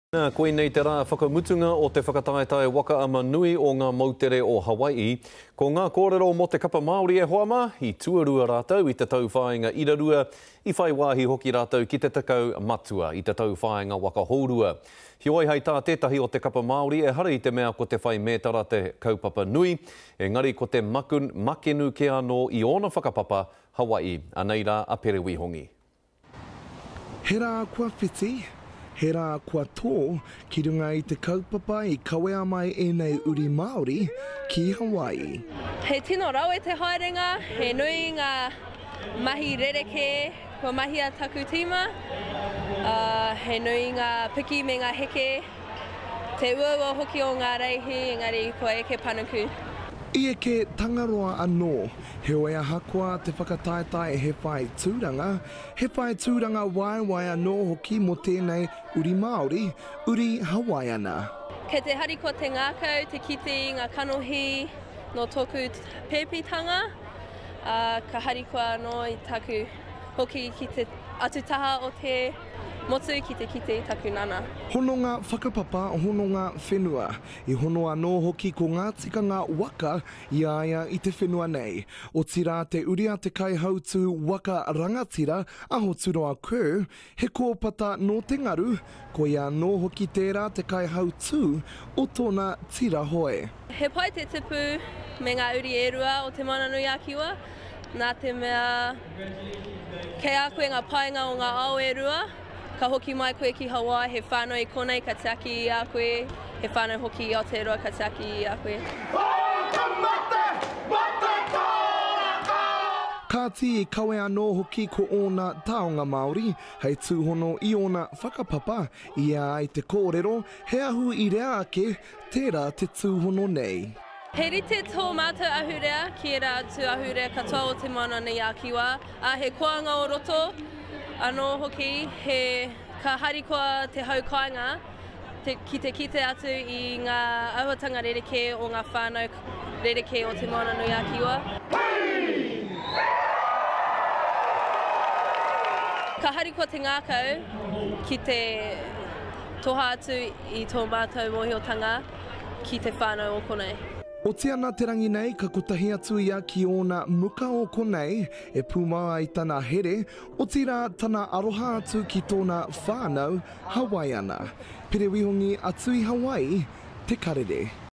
Tainui woman with Hawaiian whakapapa reconnects with her Hawaiian whānau
Today the annual Queen Lili'uokalani Long Distance Canoe Race  wraps for another year. Our Māori team placed second in the mixed division, and secured a top 10 placing in the double hull. But among the team is a young girl whose experience at the races was more than returning with a medal, but returning back to her Hawaiian roots. Pere Wihongi reports